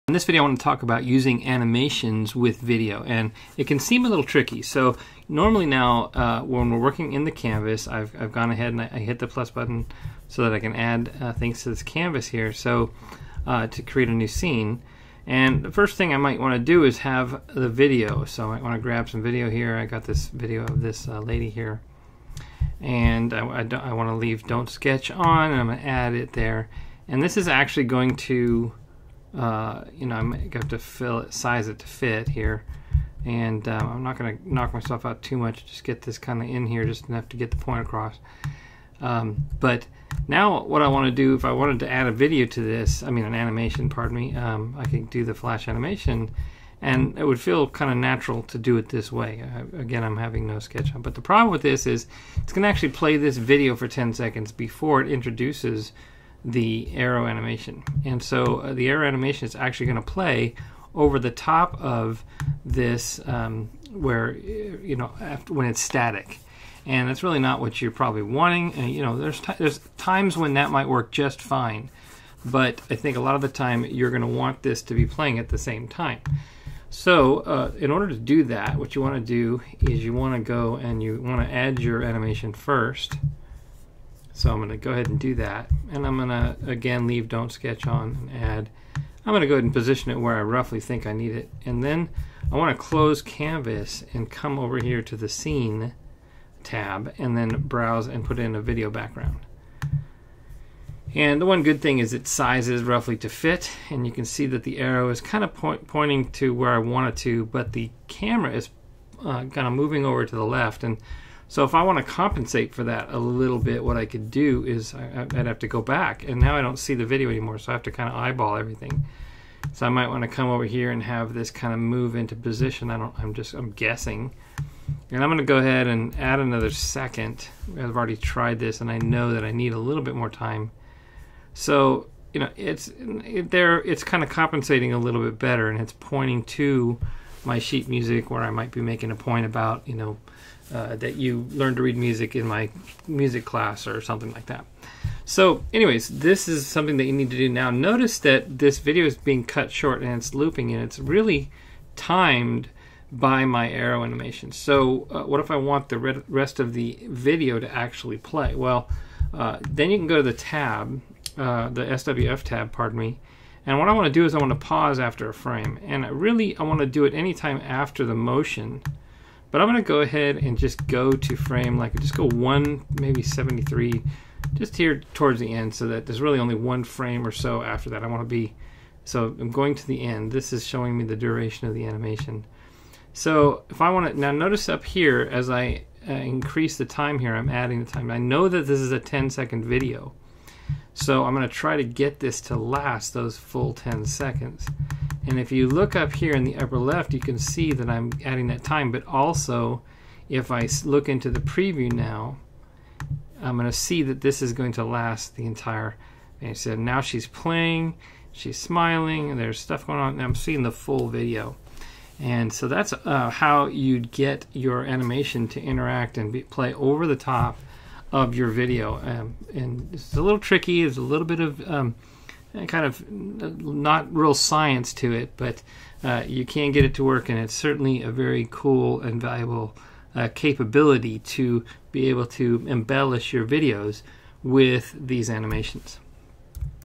Using SWF Animations Over Video In Explaindio 2
In this tutorial I demonstrated the issues and work arounds with using SWF animations over video using a sample animation from Animation Mega Foundry -- The more straightforward way of adding SWF animations with Video doesn't work out quick right. This video shows you how to get things working smoothly when embellishing video with SWF animations.